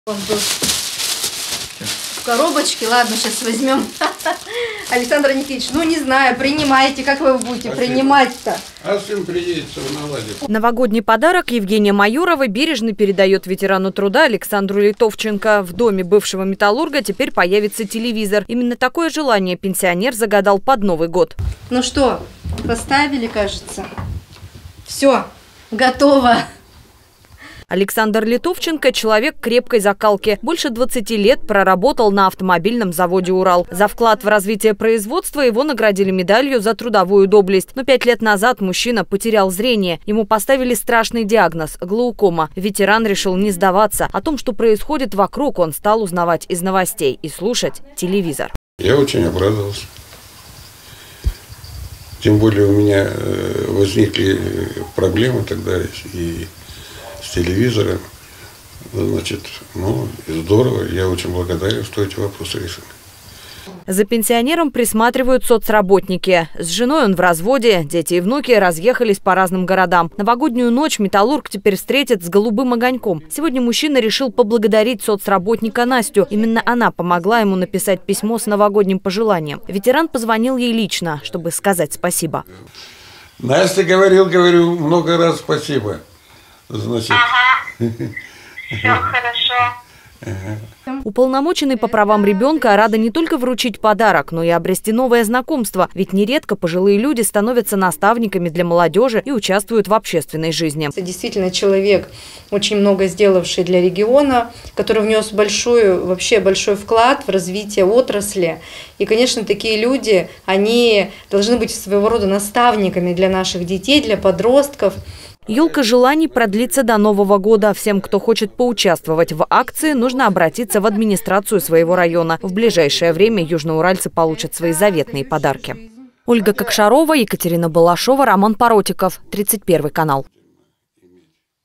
Под новый год мечты сбываются. Челябинскому ветерану труда подарили телевизор
По очень доброй традиции в конце года началась акция "Елка желаний". Ее открыл президент. Вслед за ним по всей стране желания стали исполнять чиновники. На специально установленных елках дети и пожилые люди оставляют записки, помещая их в елочные шары. Один такой шар выбрала уполномоченный по правам ребенка Евгения Майорова.